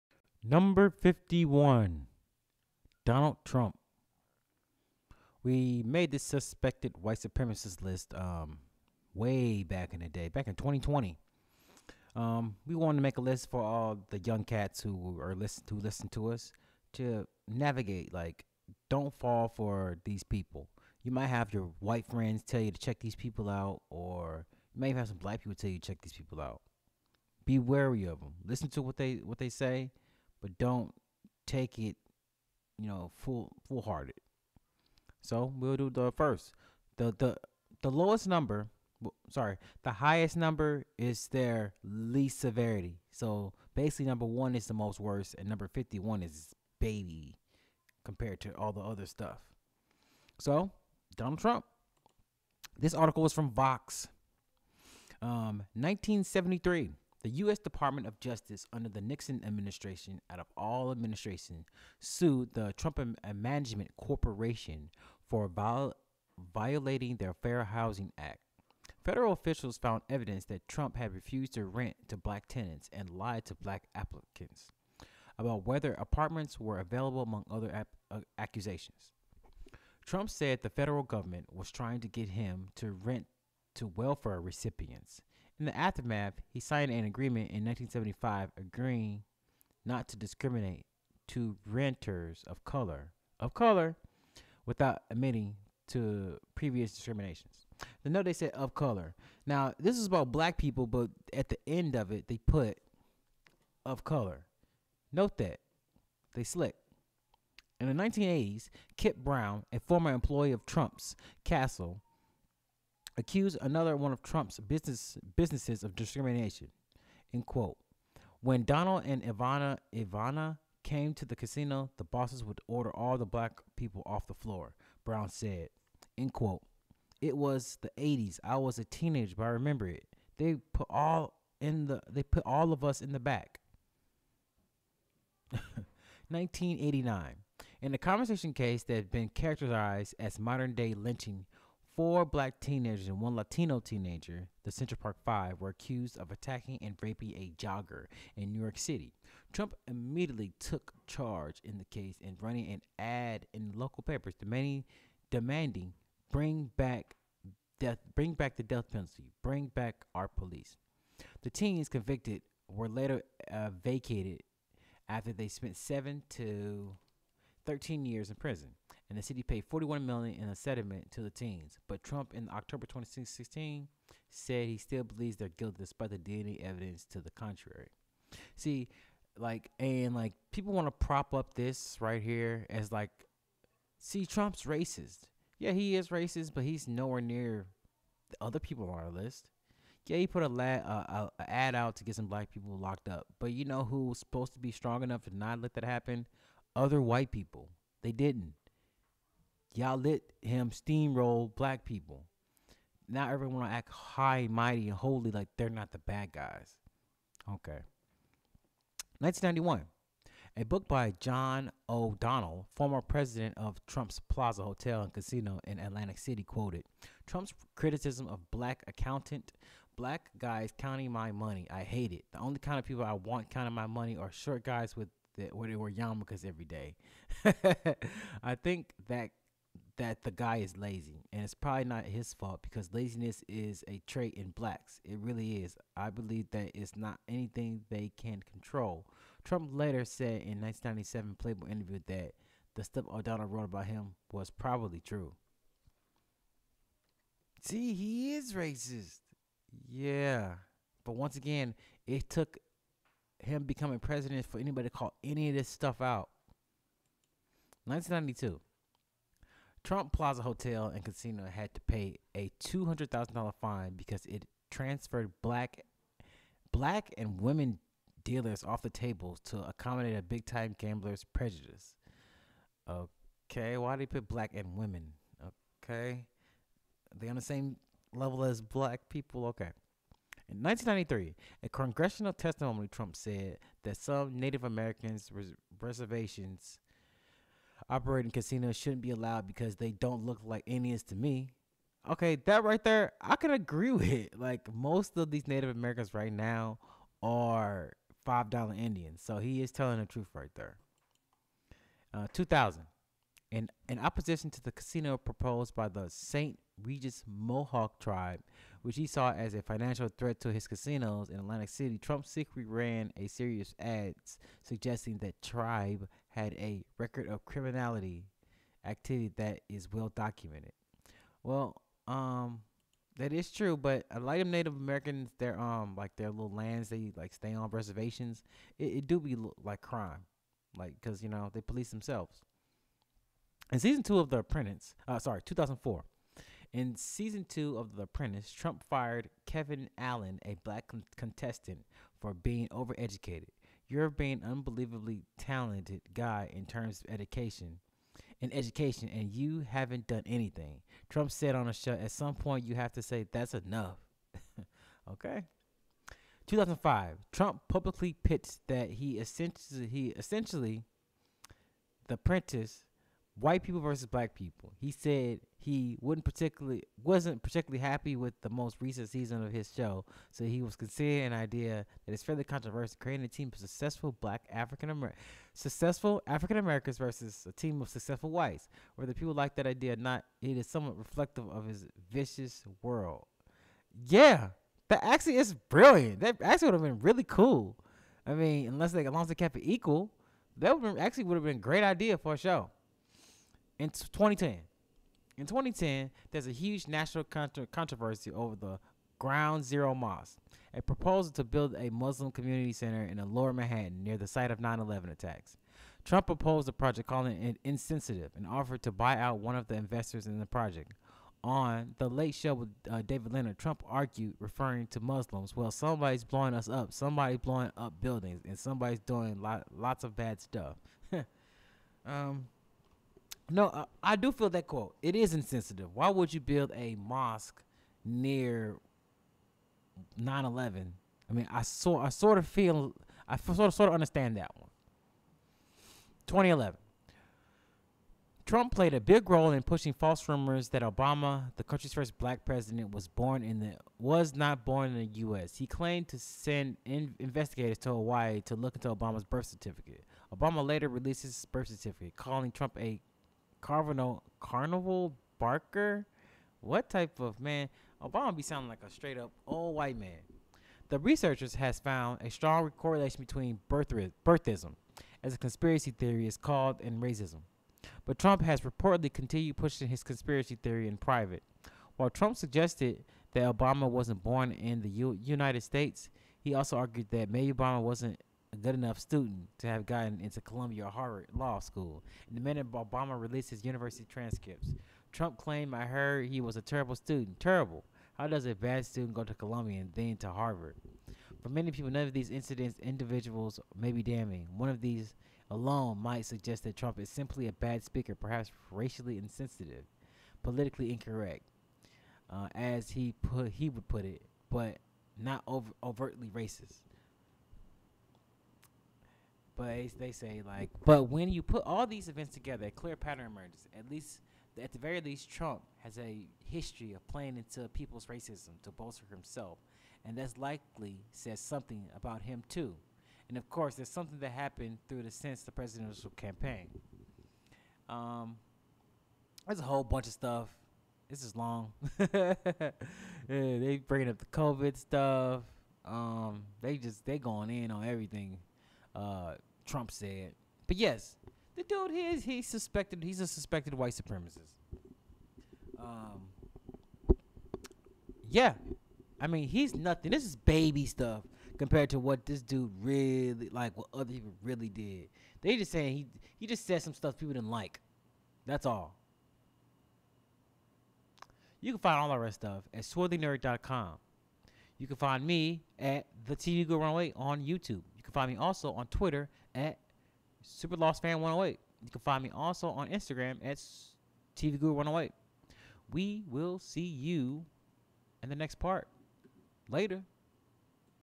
Donald Trump ~ SWS#51
Trump at the bottom of our list of Suspected White Supremacists might shock those who think his open racism makes him the worse.  However, compared to others, he's not. It's better to deal with an open SWS than a closeted one...

    Find us @

 Hit us up at

 (Stuff we talked about)